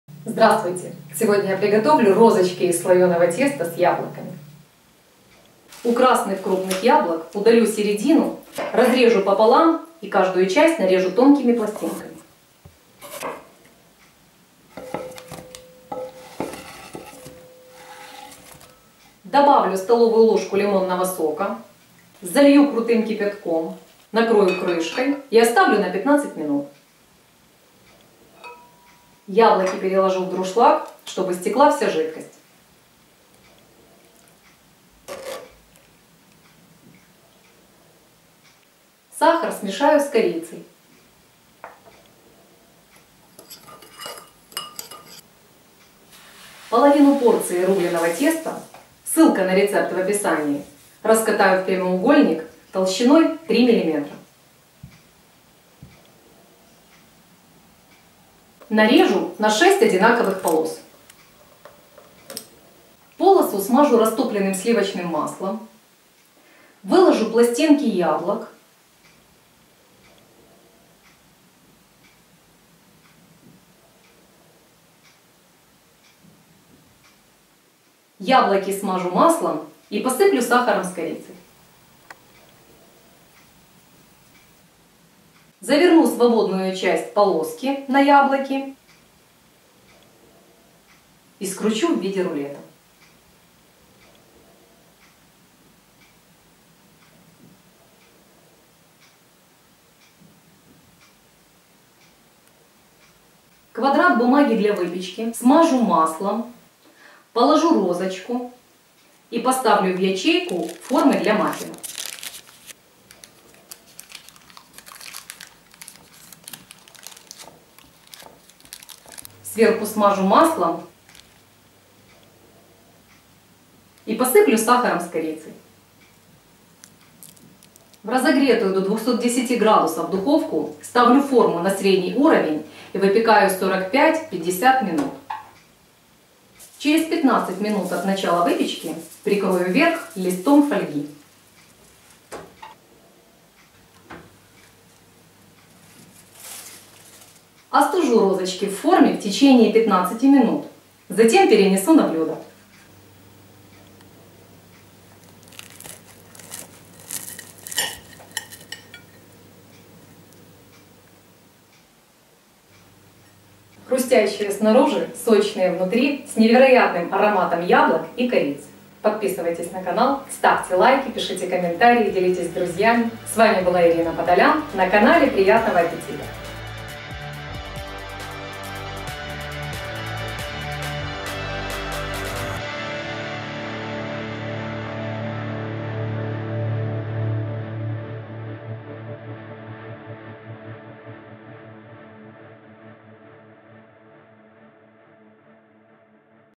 "Розочки" из слоеного теста с яблоками. Красиво и вкусно.
Оригинальная форма выпечки из слоеного теста с яблоками.
Для этого рецепта рекомендую брать красные яблоки, с другими  конечный продукт выглядит хуже.

Обратите внимание, что время выпекания может отличаться от заявленного в видео. Это зависит от размера формы, материала, из которого форма изготовлена, и особенностей духовки.

Выход - 6 штук
Ингредиенты:
тесто рубленое слоеное - 300 г
яблоки красные - 300-350 г
сахар - 60-70 г
корица - 2-3 г
масло сливочное - 50 г
лимонный сок - 15 г

Выпекать при температуре 210 С - 45-50 минут.